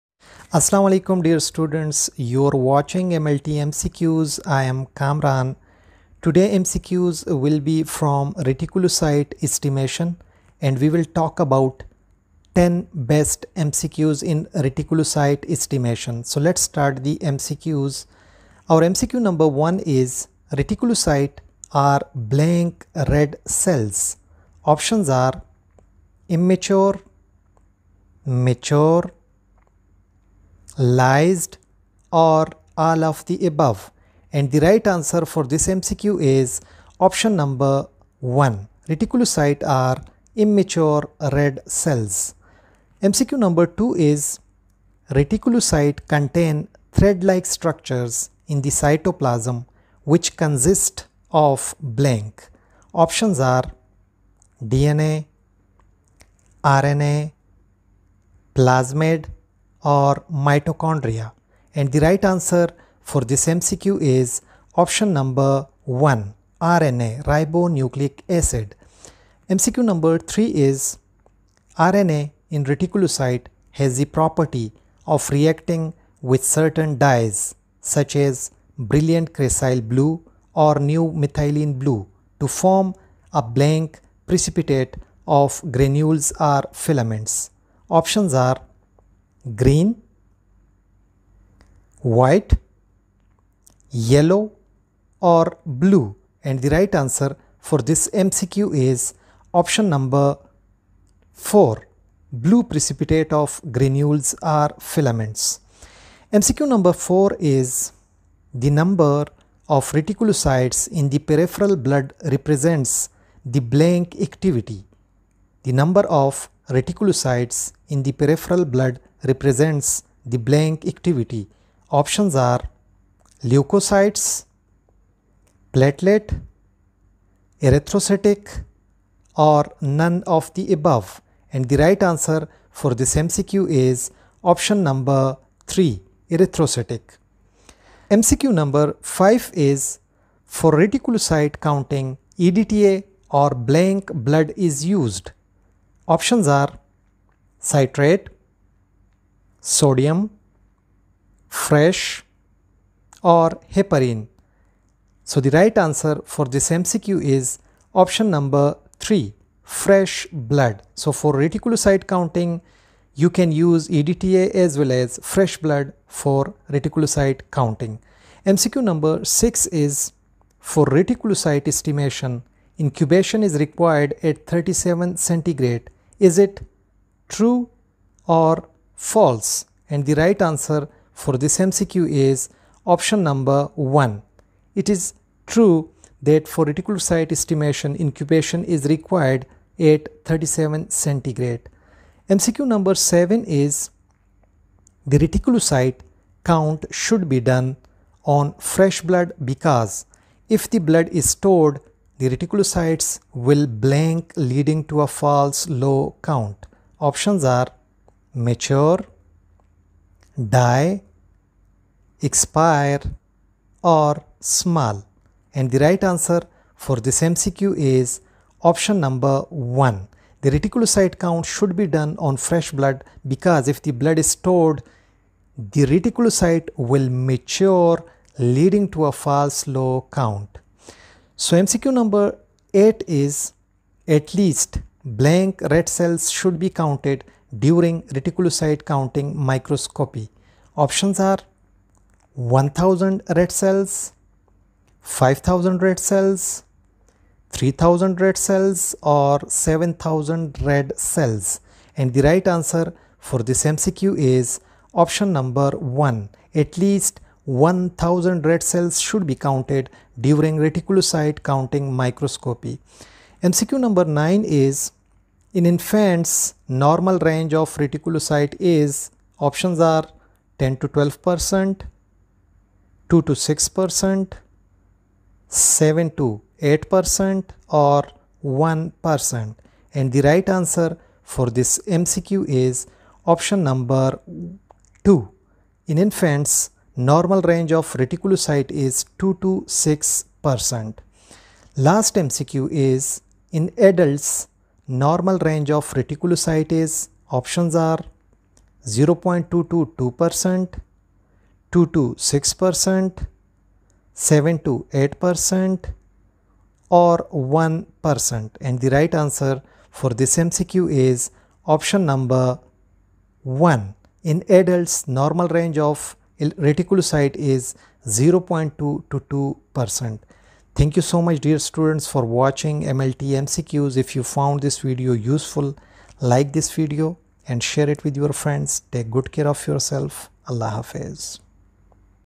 Reticulocyte Counting MCQs | In English | Top 10 MCQs in Retic Counting | Hematology MCQs | MLT MCQs
Reticulocytes are immature red blood cells (RBCs). In the process of erythropoiesis (red blood cell formation), reticulocytes develop and mature in the bone marrow and then circulate for about a day in the blood stream before developing into mature red blood cells. Dear Students and Viewer's in this video Best 10 MCQs are taken from Reticulocyte Estimation.

Queries solved in this video:
00:00 | Introduction
00:19 | MCQ 1
00:43 | MCQ 2
01:10 | MCQ 3
01:46 | MCQ 4
02:18 | MCQ 5
02:49 | MCQ 6
03:12 | MCQ 7
03:52 | MCQ 8
04:24 | MCQ 9
04:52 | MCQ 10